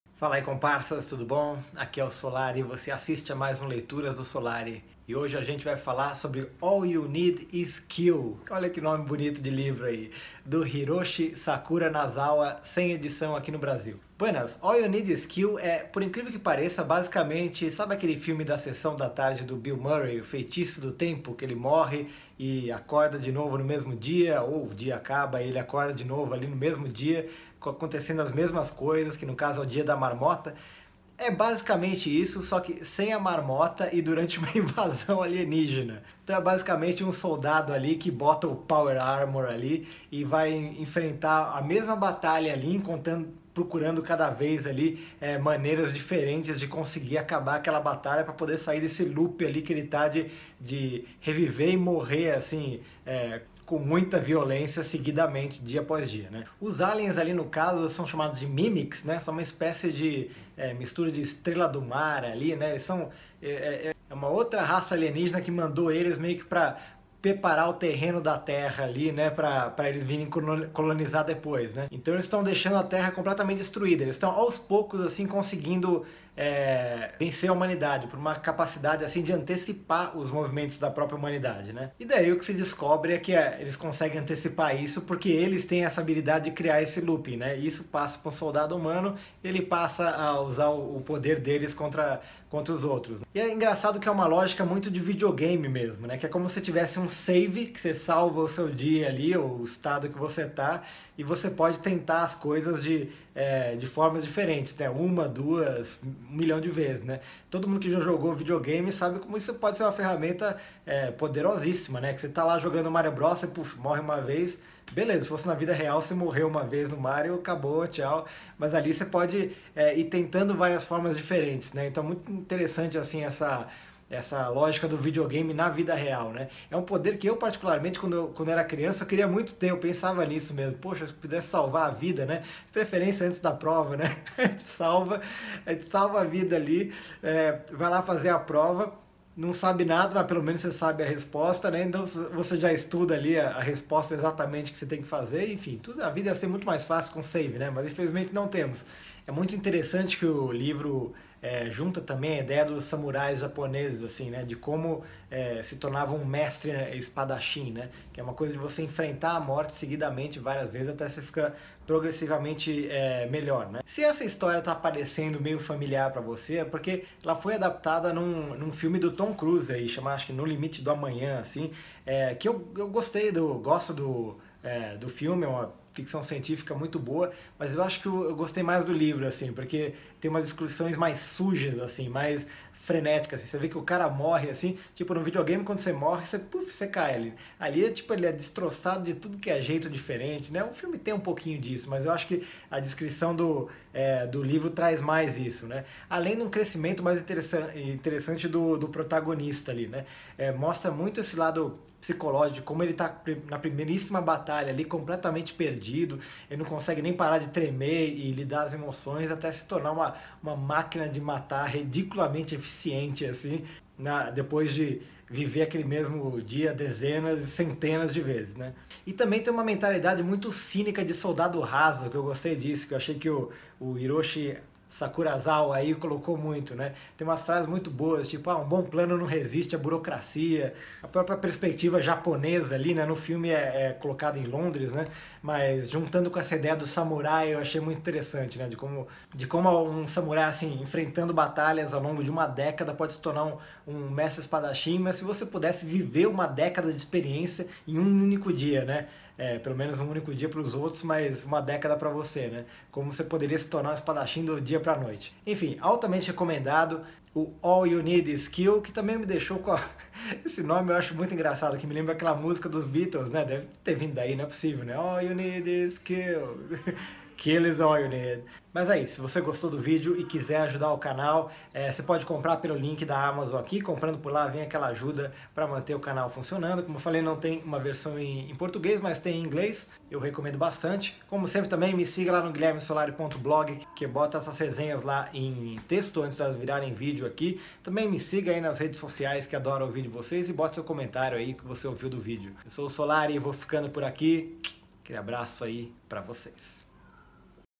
All You Need is Kill, de Hiroshi Sakurazaka 📖 Leituras do Solari #149
UPDATE: Algo que esqueci completamente de comentar no vídeo, também tem um senhor de um mangá baseado na história do livro. Não li, mas altamente recomendado pelos comparsas nos comentários e teve edição no Brasil pela JBC.

All You Need is Kill, de Hiroshi Sakurazaka, é basicamente a versão japonesa daquele filme do Bill Murray o Feitiço do Tempo, em que ele revive o mesmo dia seguidamente, todo dia. Só que aqui é um soldado que todo dia revive uma batalha sanguinária contra uma invasão de aliens semelhantes a estrelas do mar assassinas. É uma batalha que a humanidade está lenta, mas constantemente, perdendo, porque os aliens curiosamente parecem sempre antecipar os movimentos dos exércitos humanos.

Até que um soldado consegue reviver a batalha todo dia como se estivesse jogando um videogame. Assim que ele morre, ele volta para o “save” dele e pode tentar tudo novamente. Uma, duas, uma centena de vezes. E é fascinante as possibilidades de ter basicamente um botão de “save” para a vida, poder fazer uma baita burrada e carregar o “jogo” de onde estava. Eu lembro que quando era mais novo pensava nisso, para motivos menos nobres do que salvar a humanidade, mas que tal fazer a prova, bombar feio, mas voltar no “jogo” já sabendo as questões?

Vai comprar o livro e quer ajudar o canal? Você pode comprar através do link abaixo que você colabora a manter o canal funcionando.

All You Need is Kill